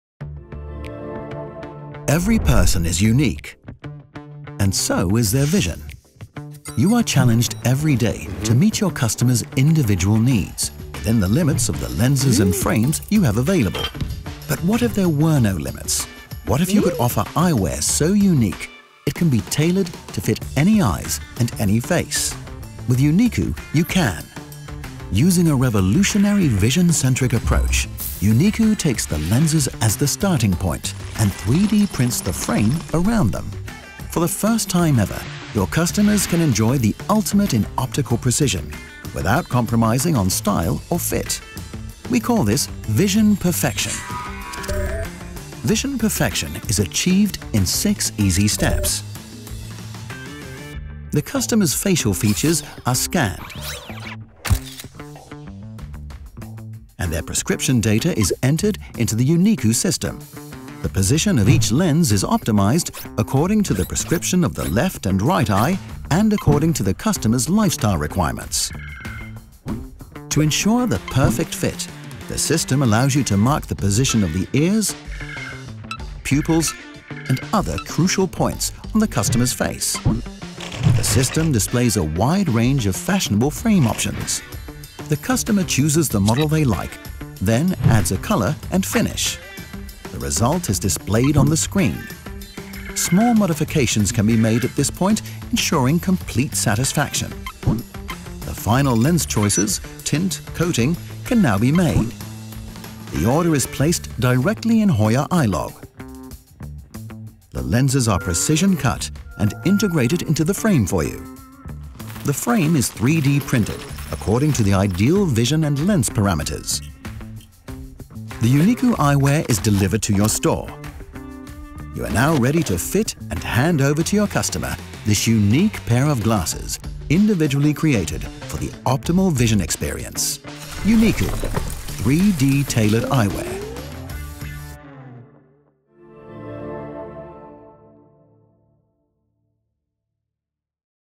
Yuniku process animation by Hoya
Yuniku 3D tailored eyewear is designed entirely around your face and your vision. Regular spectacles can be customised only to a certain degree. The frame and fitting are a given, and the lenses have to be adjusted to suit them. Yuniku, by contrast, uses a revolutionary vision-centric approach. It begins by assessing your visual needs and facial features. Advanced software calculates the ideal position of the lenses in relation to our eyes, and then 3D prints the frame based on those unique parameters. For the first time, you can enjoy the ultimate in optical precision, without compromising on style or fit.